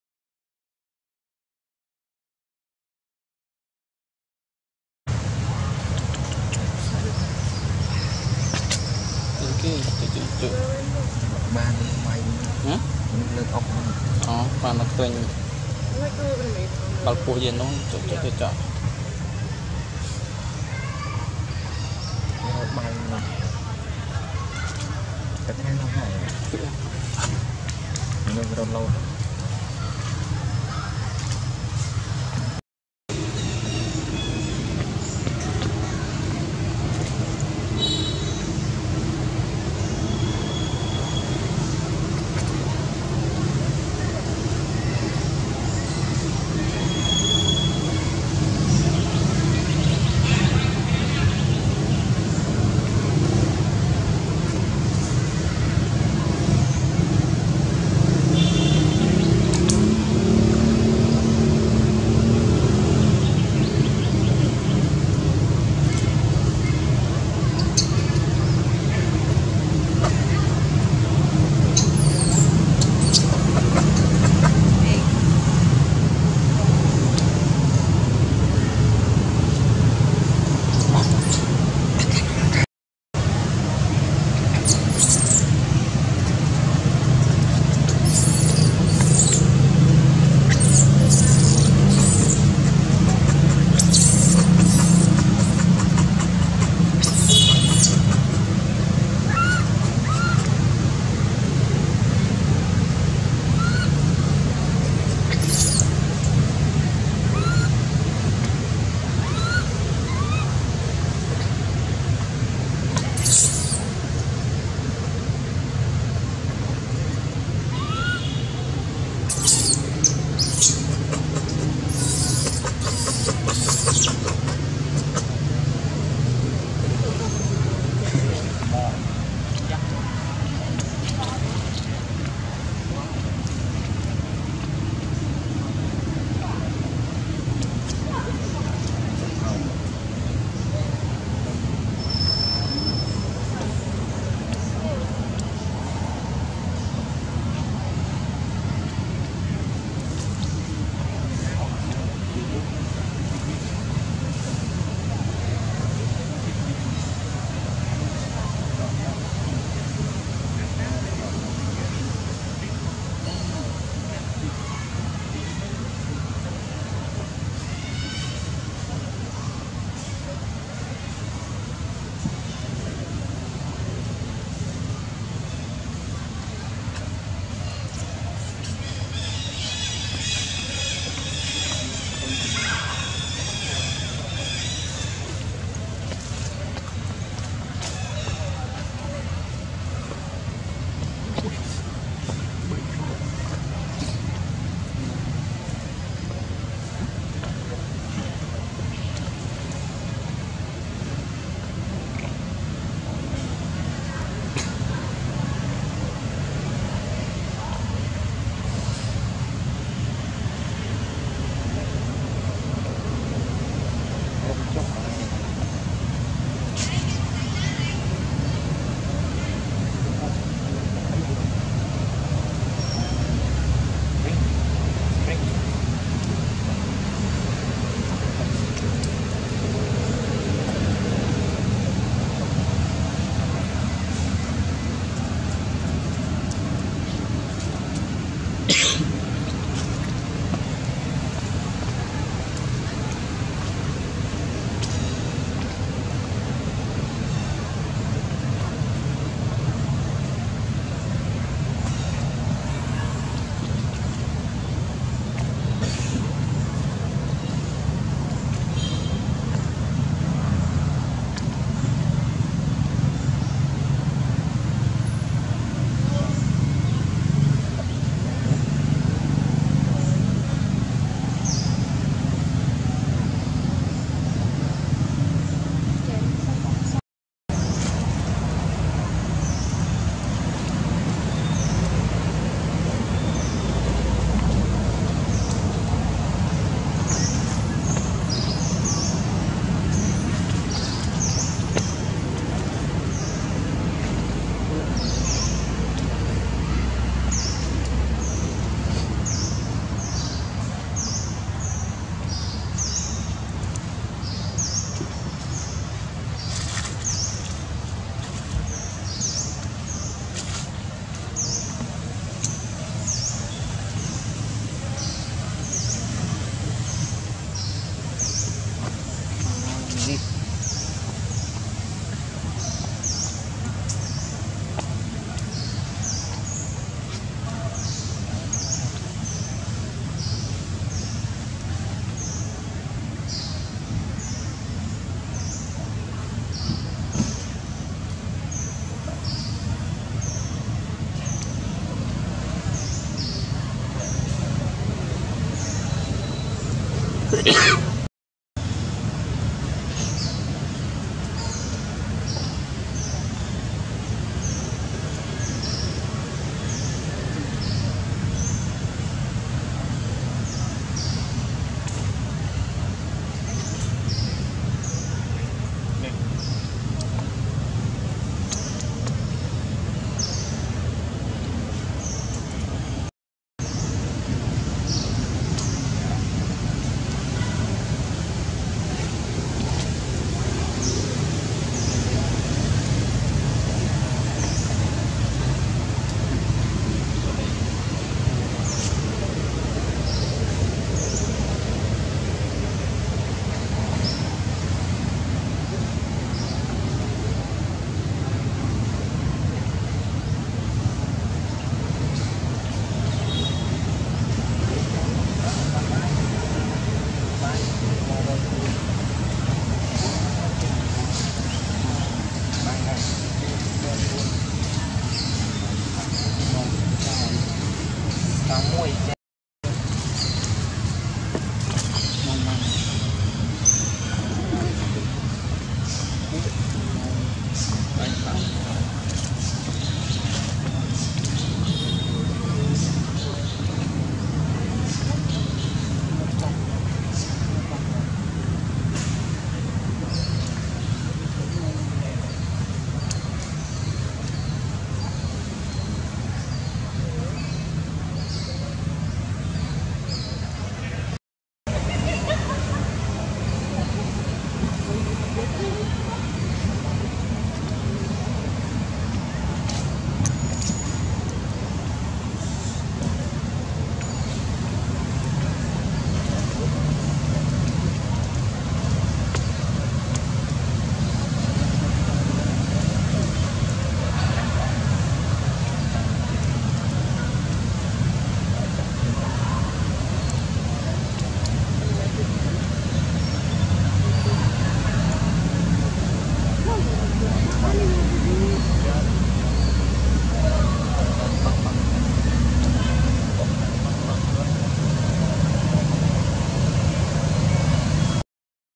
Why Baby Janna Cries And Screams?
Hello! Everyone Please Watch Baby Janna Cries And Screaming And Then Take To Other Place. WELIKE MONKEY